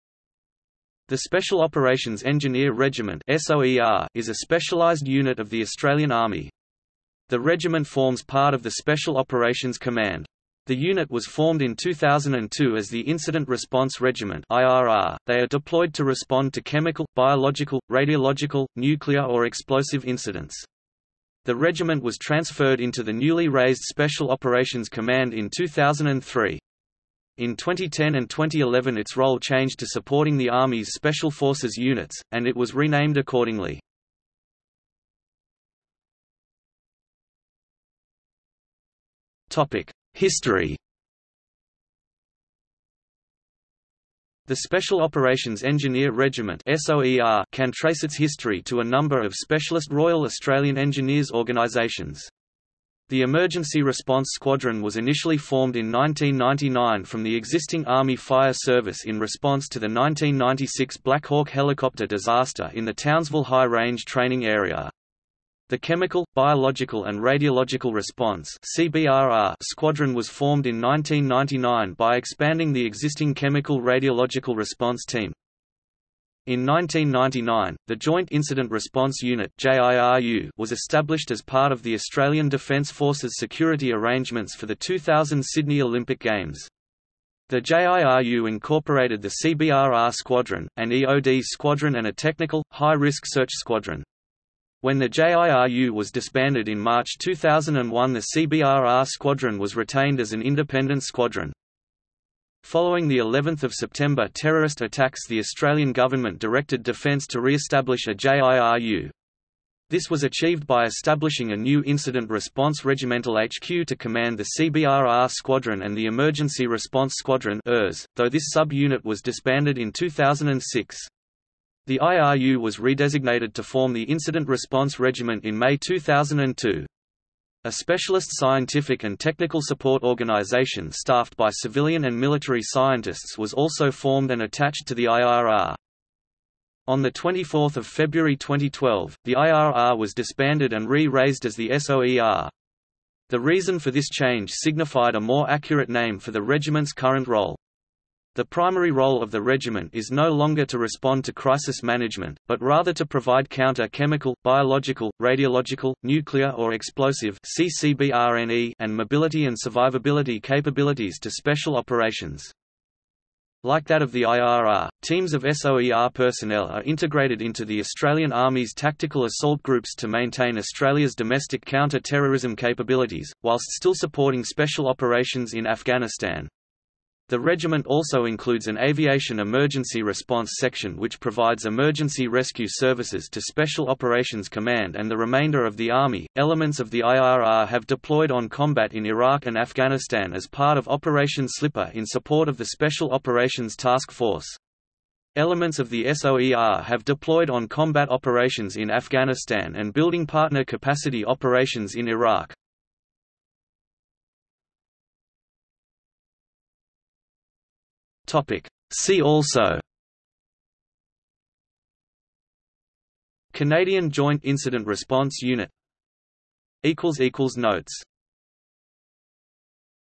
Special Operations Engineer Regiment (Australia) | Wikipedia audio article
00:00:35 1 History
00:03:24 2 See also

- Socrates

SUMMARY
The Special Operations Engineer Regiment (SOER) is a specialised unit of the Australian Army. The Regiment forms part of the Special Operations Command. The unit was formed in 2002 as the Incident Response Regiment (IRR), they are deployed to respond to chemical, biological, radiological, nuclear or explosive incidents. The Regiment was transferred into the newly raised Special Operations Command in 2003. In 2010 and 2011 its role changed to supporting the army's special forces units, and it was renamed accordingly.